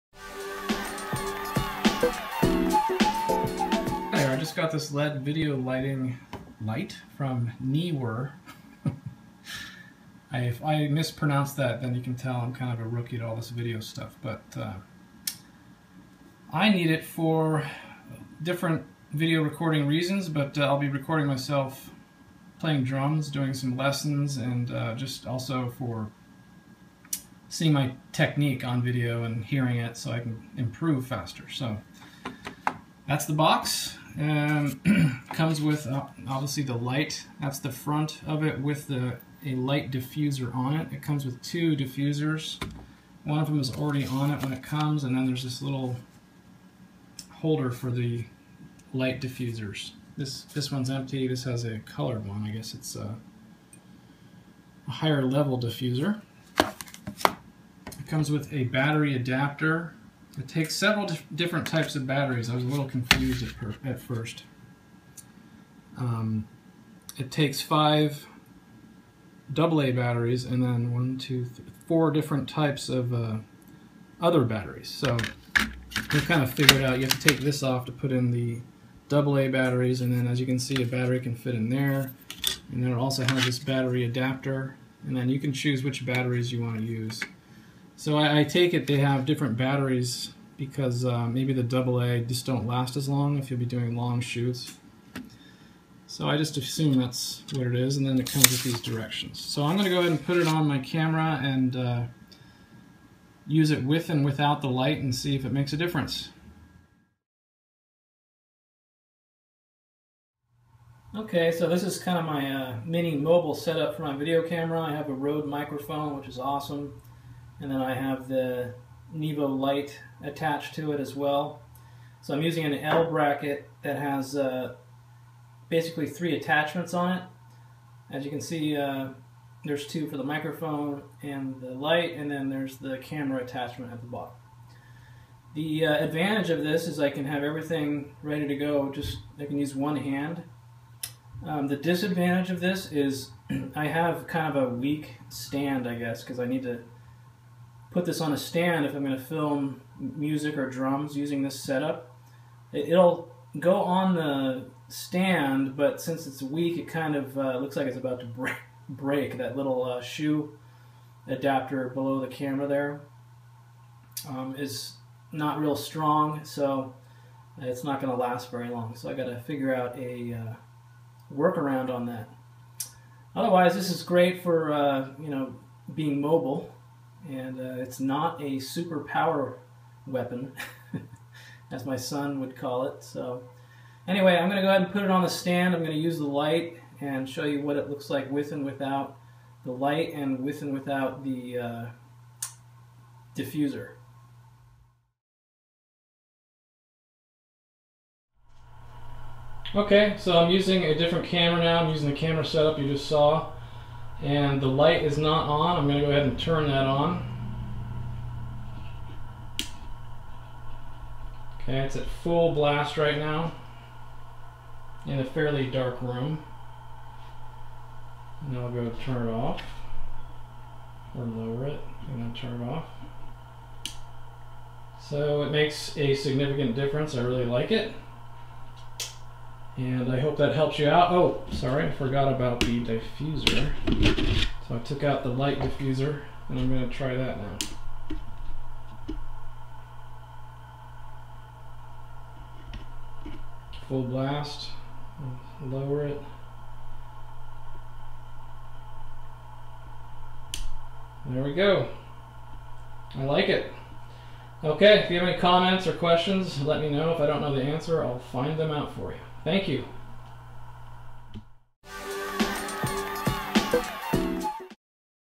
Led Video Light Review CN 160
This is a review of the cn 160 Neewer led video light. I am using it to record video myself playing drums and for video recording that requires me to be more mobile. See my set up.